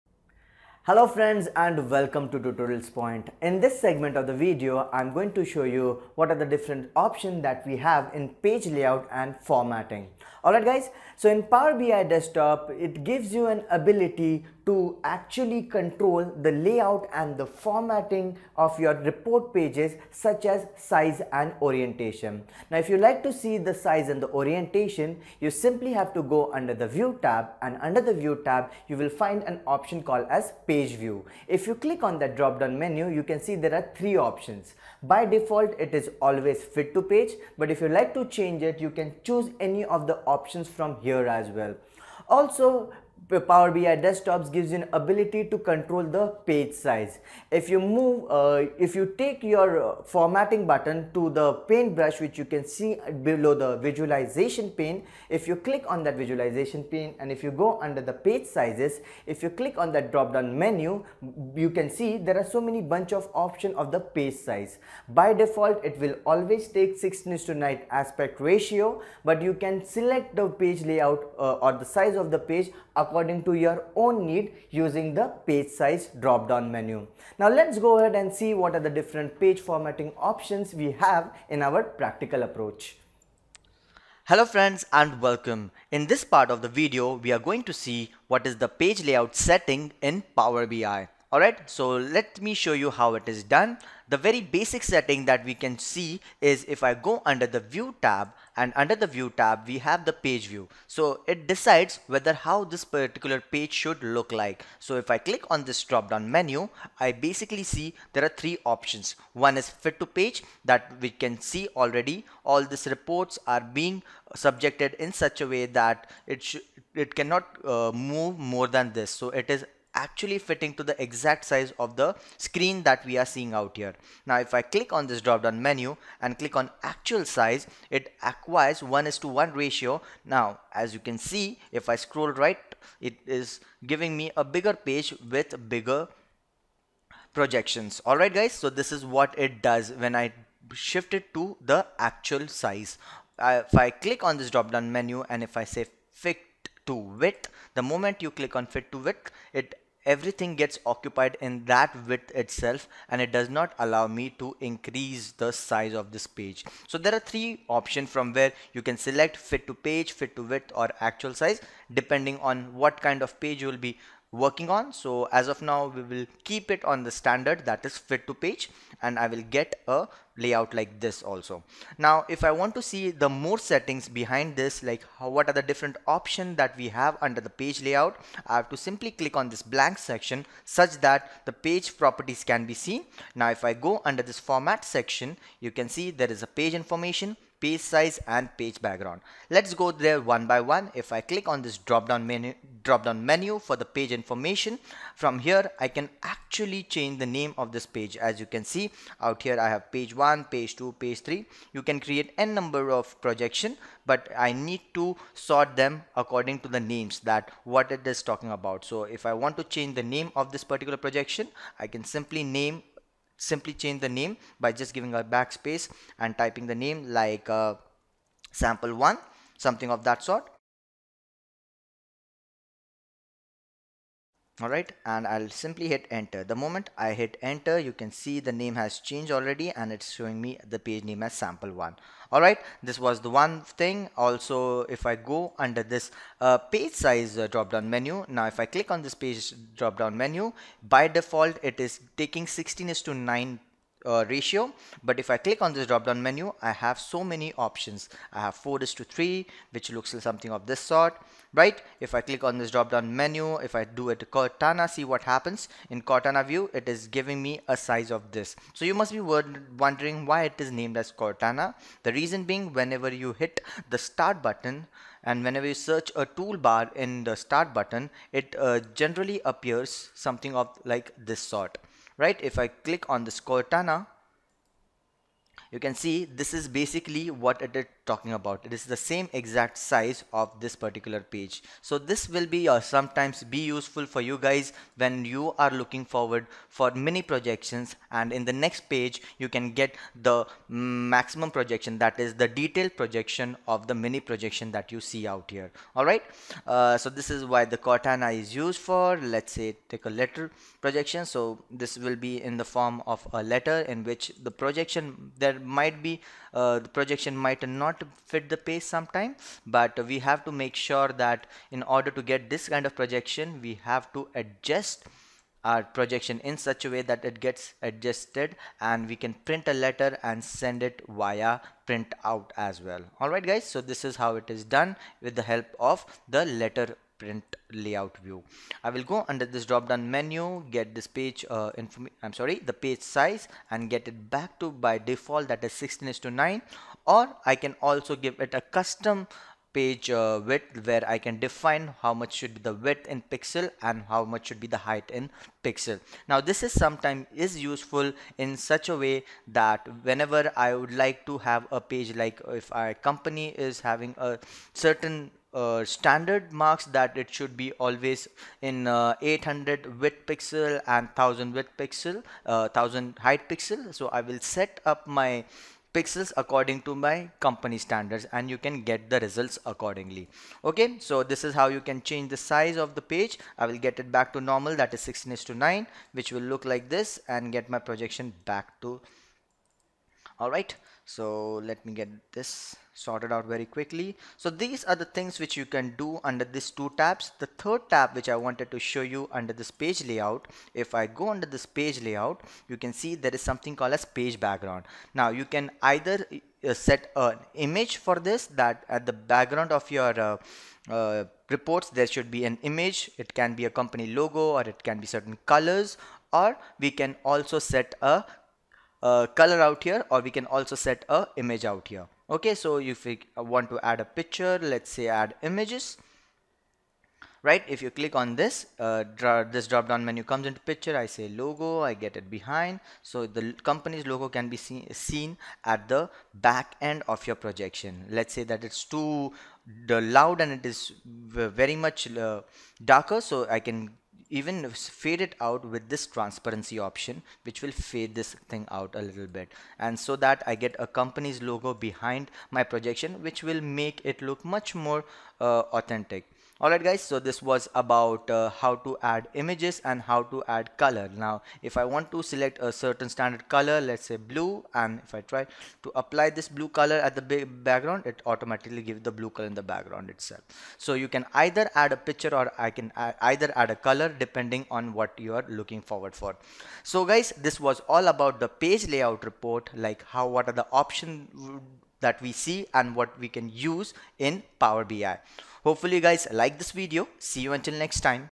Power BI - Page layout and formatting
Lecture By: Mr. Akshay Magre, Tutorials Point India Private Limited